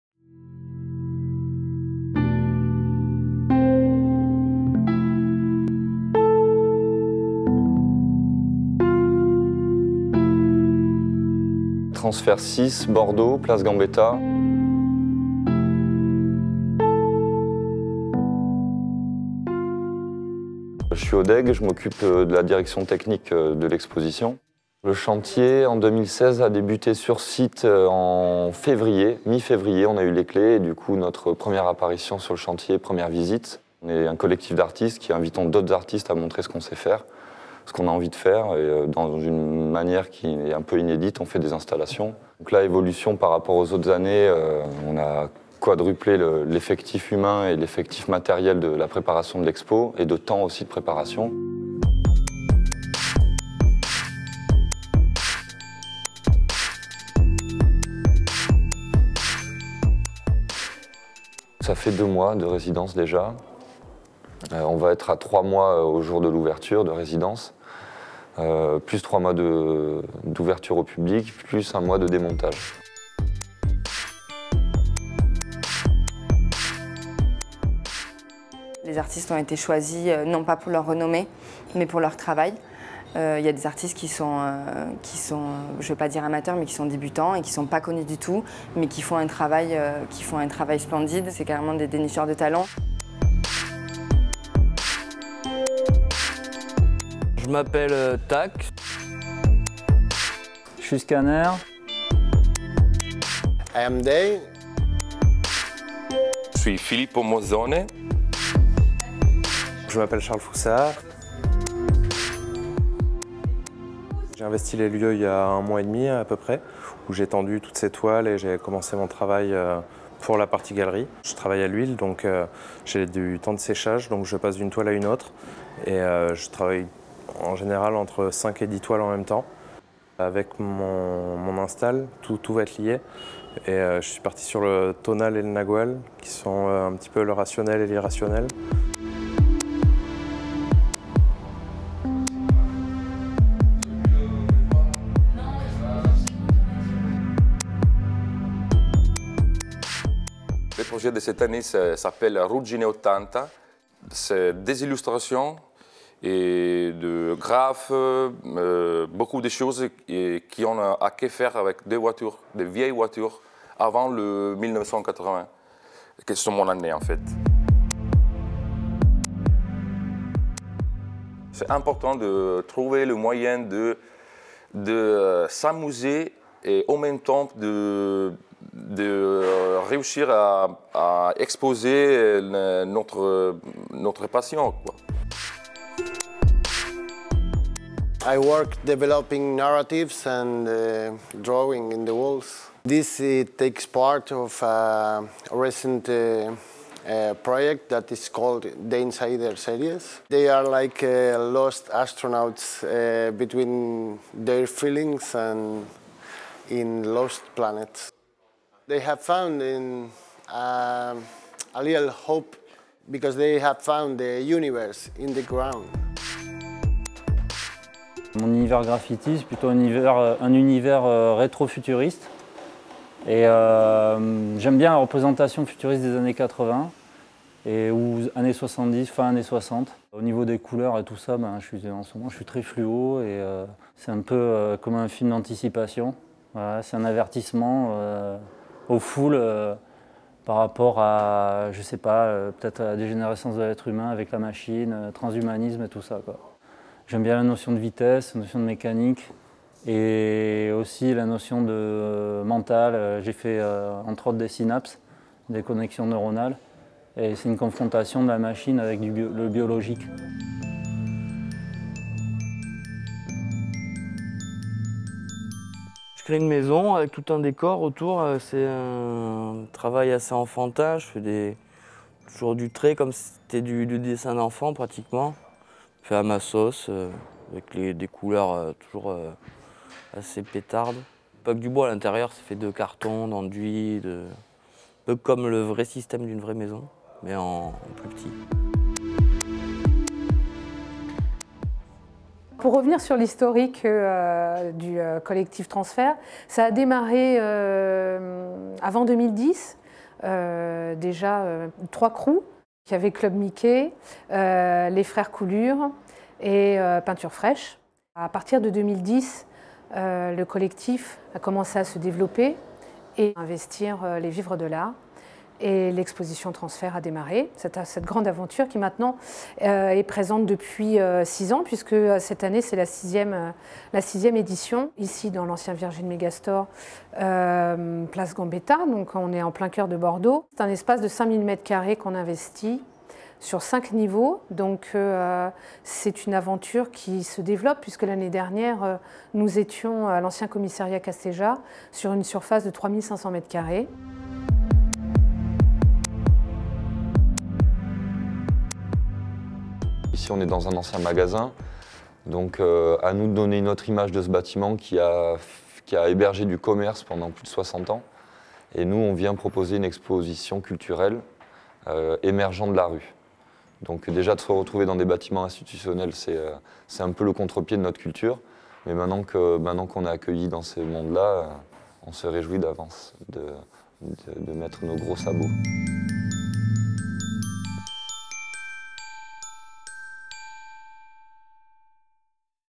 TRANSFERT #6
Retour en images sur les coulisses du montage de l’exposition TRANSFERT #6 avec Odeg, directeur technique de l'exposition, Patti Clairet et Maud Larquier, en charge de la communication de l'événement, et les artistes : Scaner, Deih, Charles Foussard, Mozone et Tack.